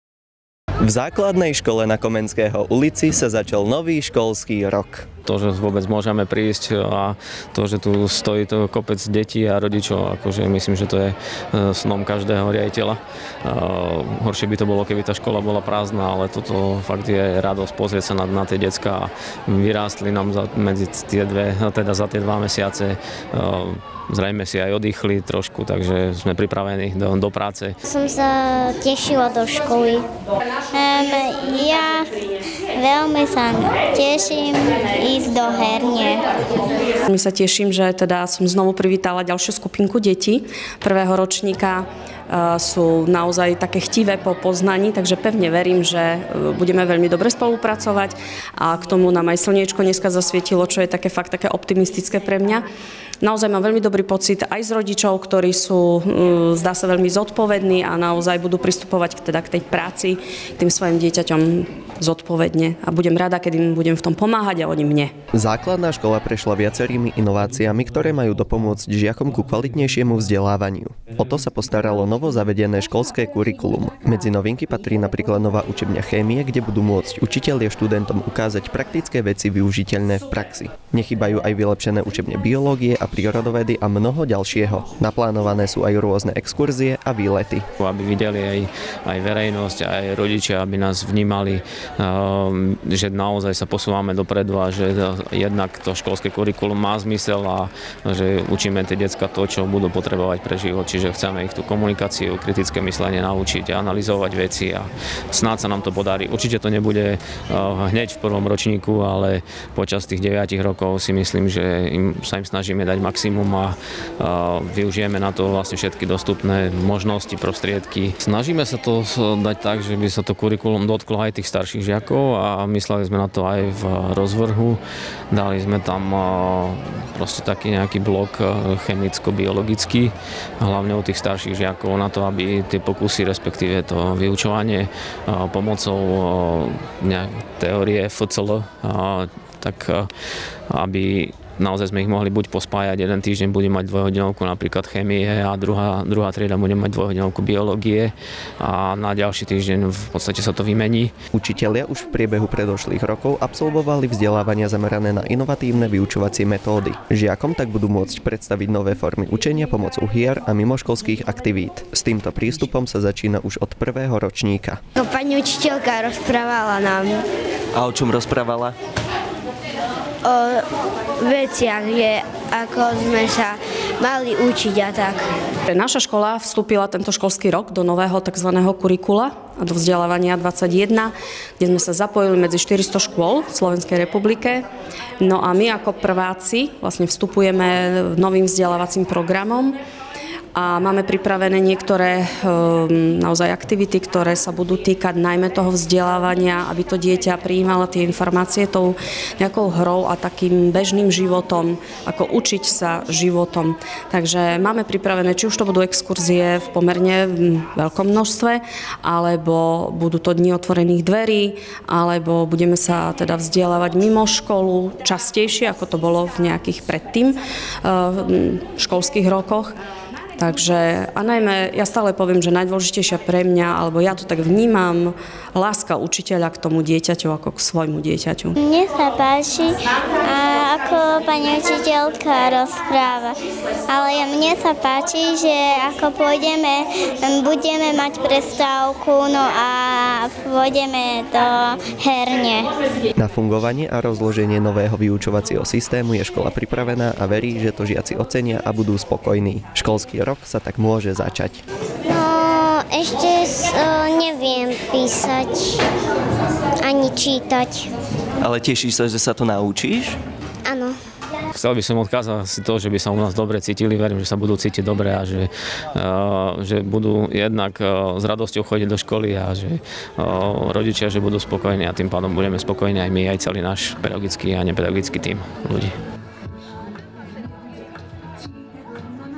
Začiatok školského roka v Základnej škole na Komenského ulici
Aj v ZŠ na Komenského ulici sa uskutočnilo otvorenie nového školského roka. Na žiakov okrem nových zážitkov a predmetov čaká aj nové školské kurikulum, ktoré pozmení vyučovací systém. K učebniam biológie a prírodovedy sa pridá nová učebňa chémie, kde budú učitelia môcť študentom ukázať praktické veci využiteľné v praxi. Pre žiakov sú pripravené exkurzie a výlety, pri ktorých sa budú učiť zábavnou formou.